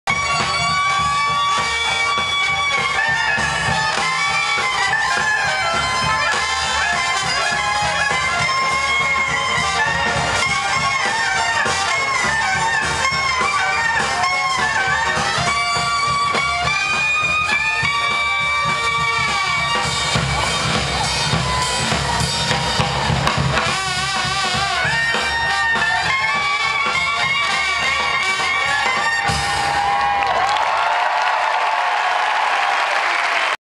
Tulane Graduation 2008, Jazz and that Fluttery Hanky Thing
The nice jazz band played, and people waved hankies.  weird.